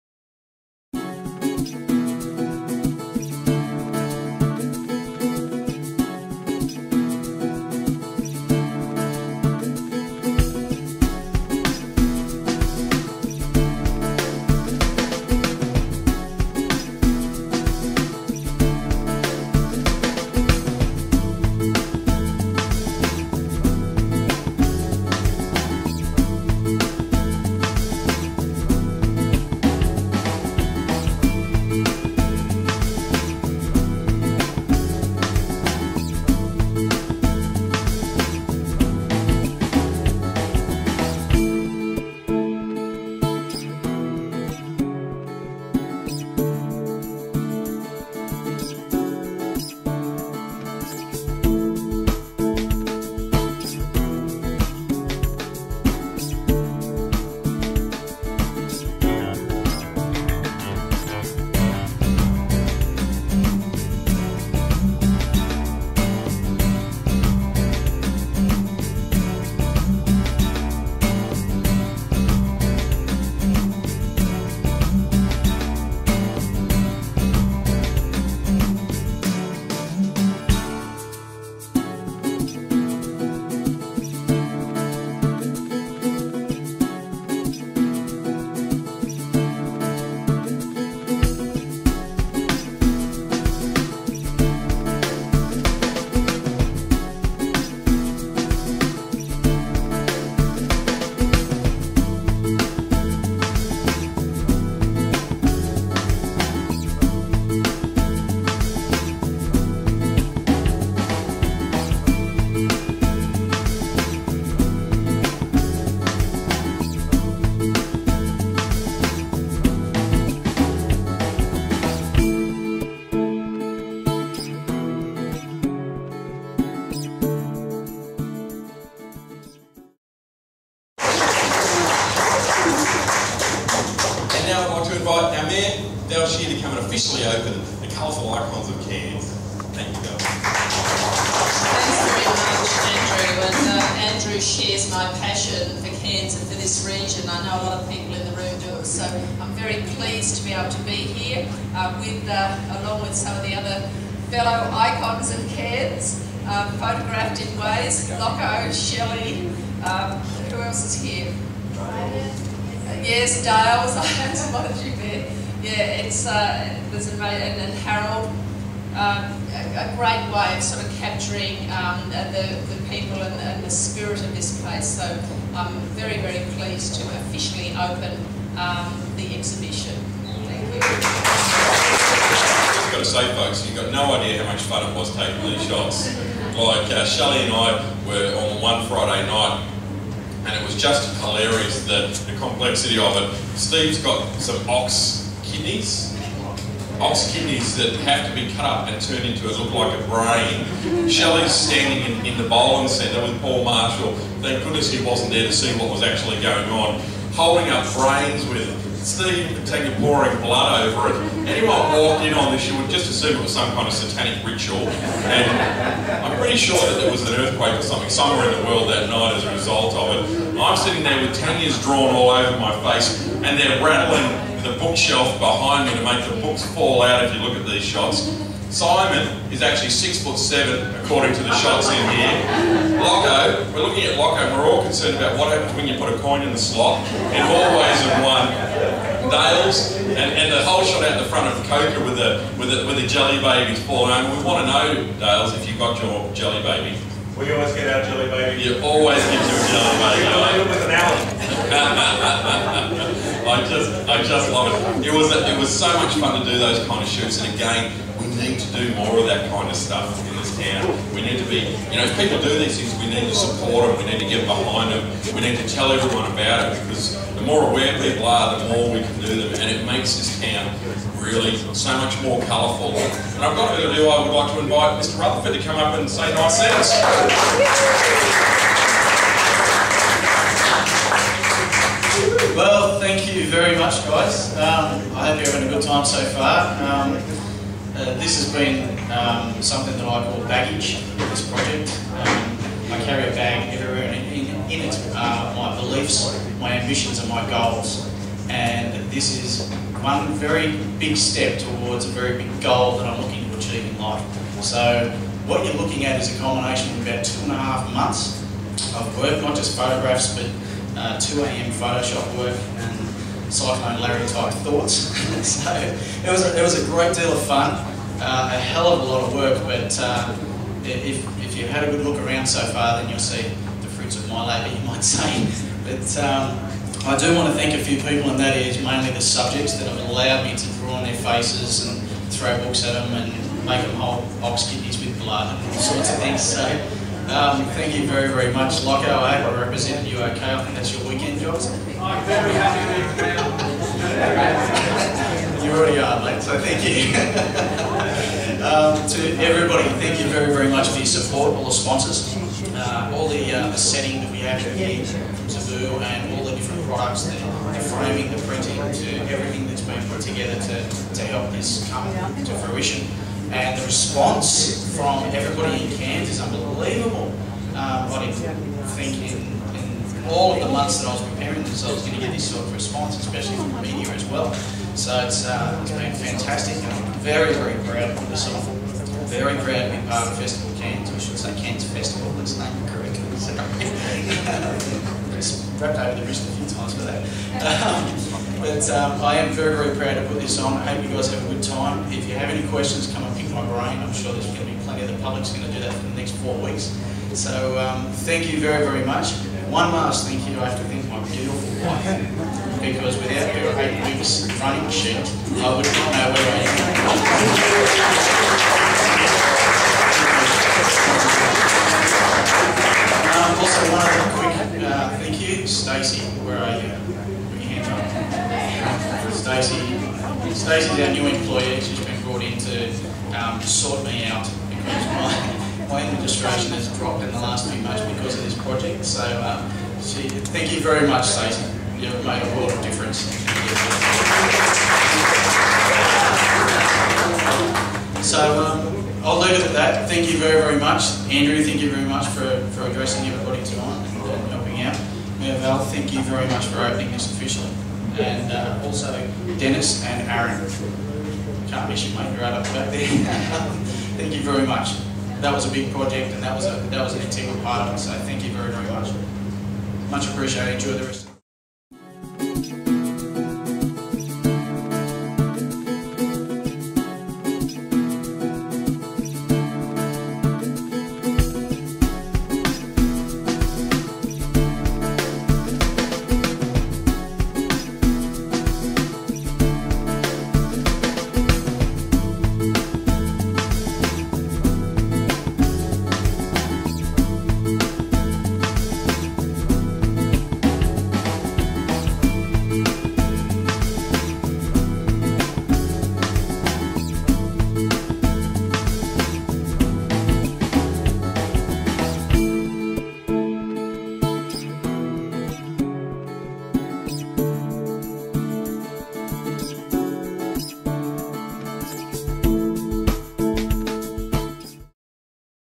Colourful Icons of Cairns
The opening night for Steve Rutherford's Colourful Icons of cairns Exhibition